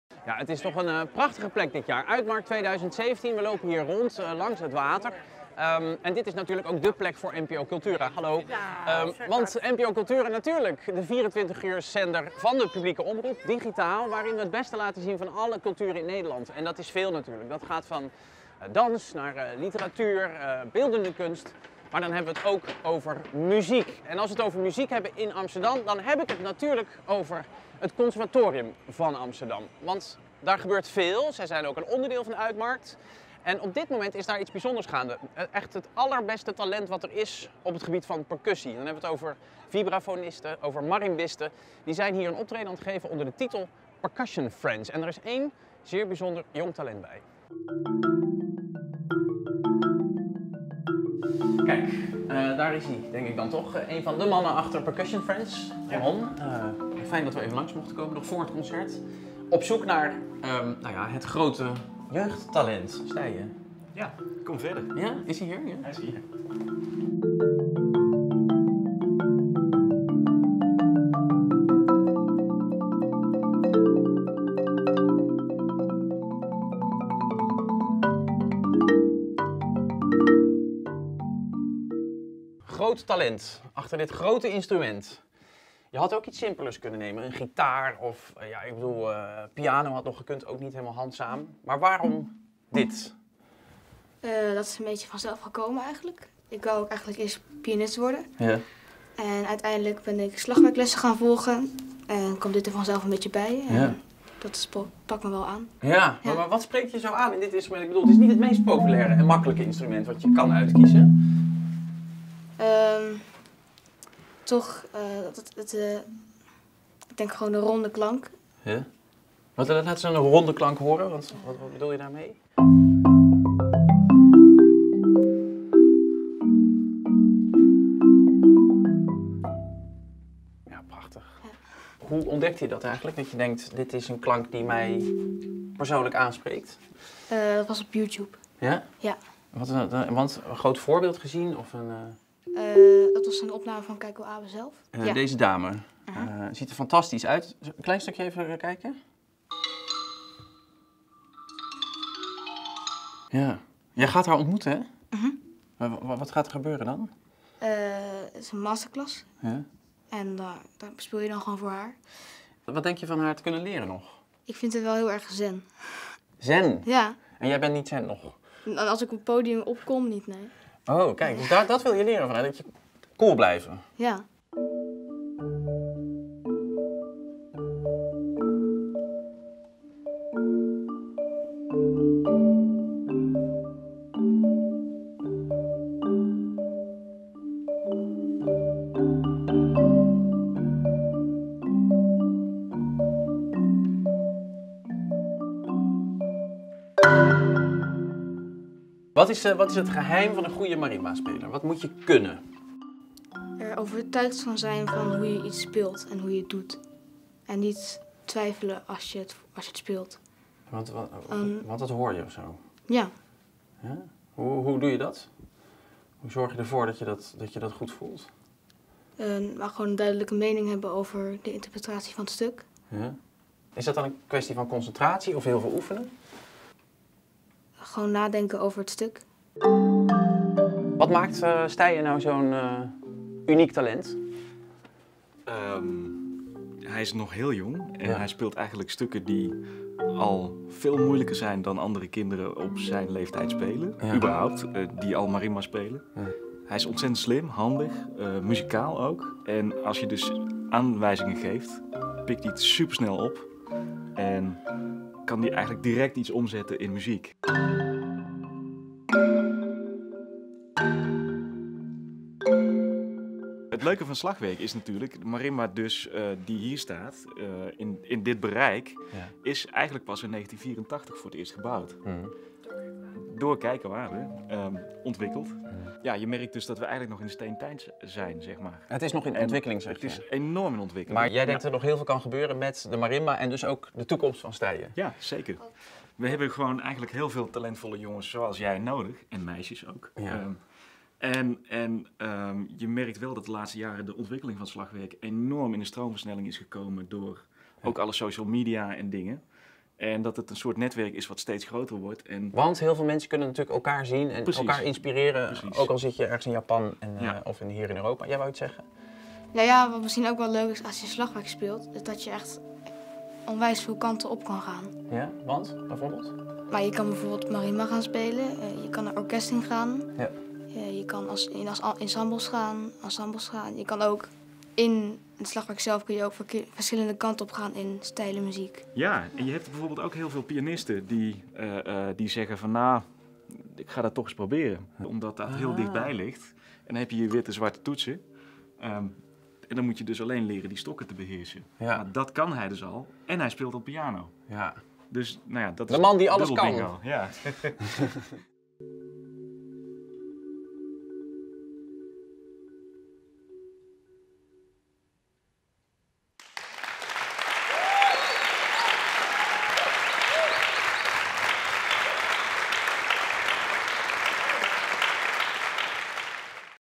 Jong percussietalent | Uitmarkt 2017
Percussion Friends is op zoek naar jong talent. Dat hebben ze gevonden in marimba-speler Steije Maurer. Percussionist Ramon Lormans neemt Winfried Baijens mee naar dit uitzonderlijke talent.